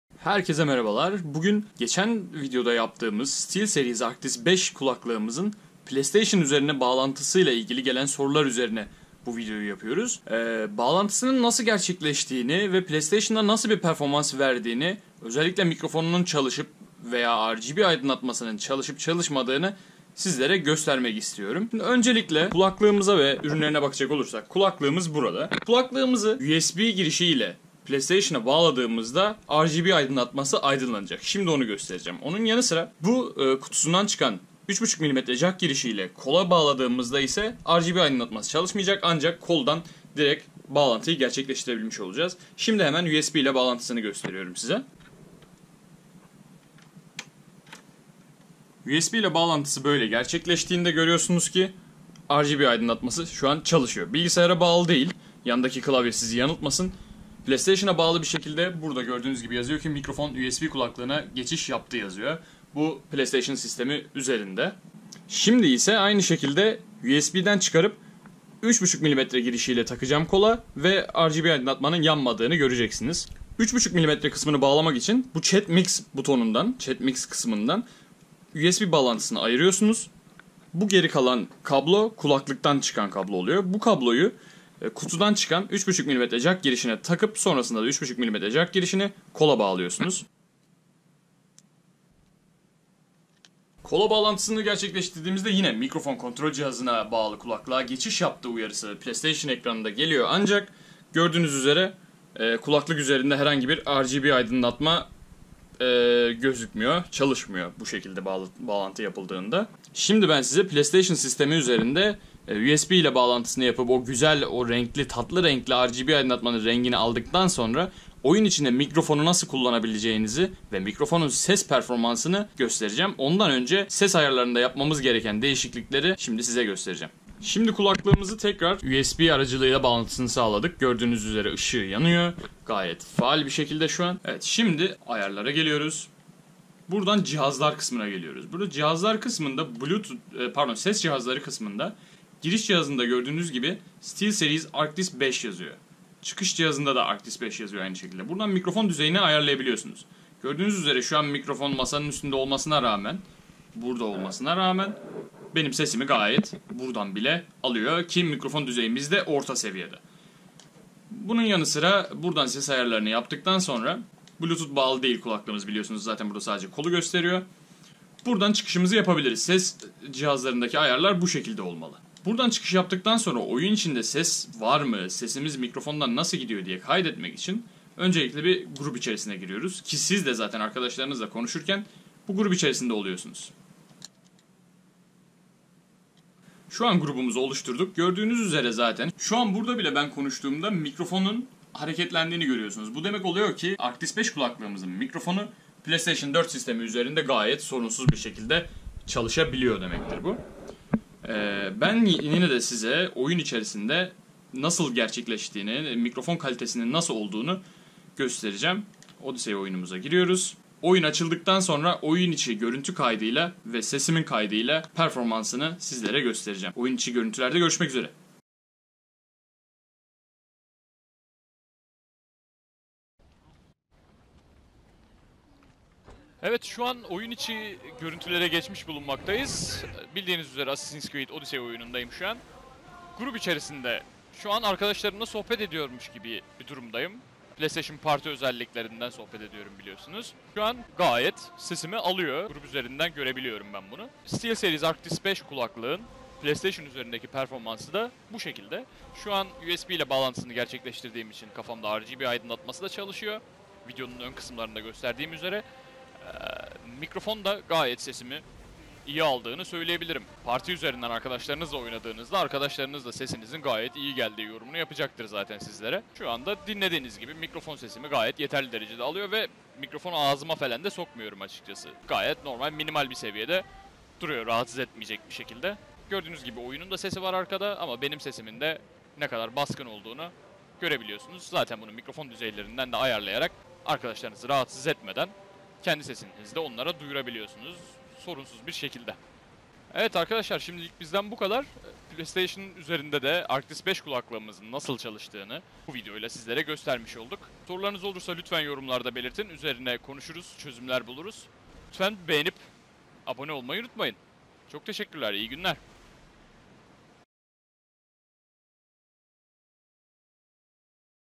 Arctis 5 // PS4 Kurulum ve Mikrofon Testi
Önceki videolarda açılımını yaptığım Arctis 5 Kulaklığın Playstation 4 üzerinde nasıl kurulacağını, RGB aydınlatmanın hangi şekilde çalışacağını ve mikrofonun kalitesini gösterdim.

Sorularınızı lütfen yorumlar kısmına yazın ki hep birlikte diğer sorulara da çözüm bulalım.

İyi Seyirler...

ekap